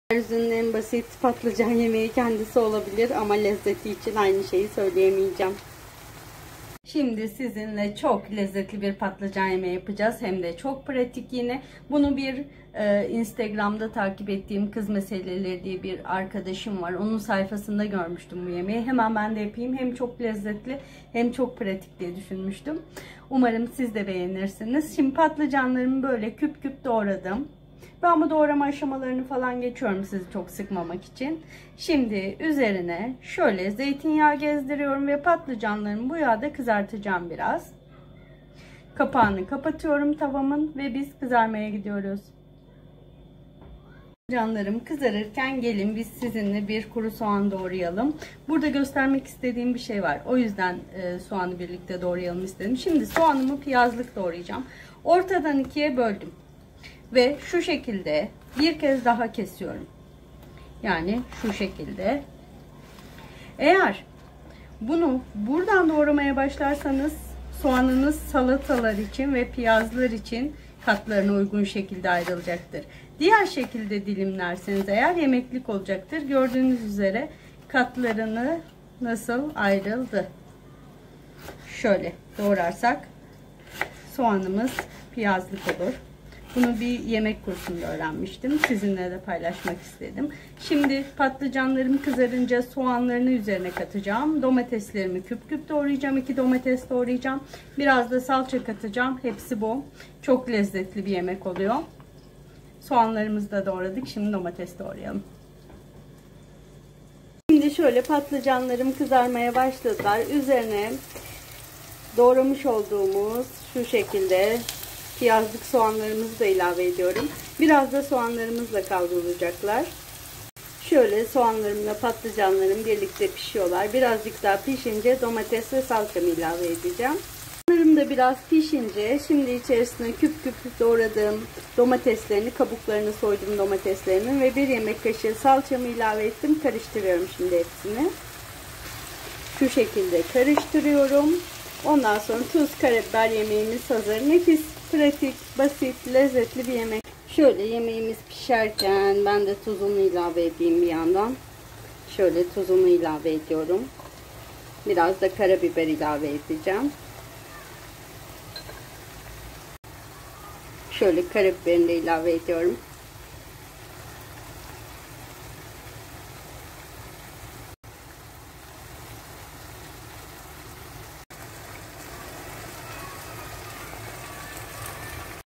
Domatesli Patlıcan 🍆🍅🍆
Tam yaz yemeği 🤩Bu kadar basit bu kadar lezzetli yemek mi olurmuş 😶Olurmuş 😀Ben de dün takip ettiğim @kizmeseleleri sayfasında gördüm bu tarifi aa ne kolay ne leziz bir tarif dedim ve hemen yaptım 🤩Iyi ki de yapmışım 🙃Siz de KAYDEDİN 😊ve deneyin 😉

📍Tuz,karabiber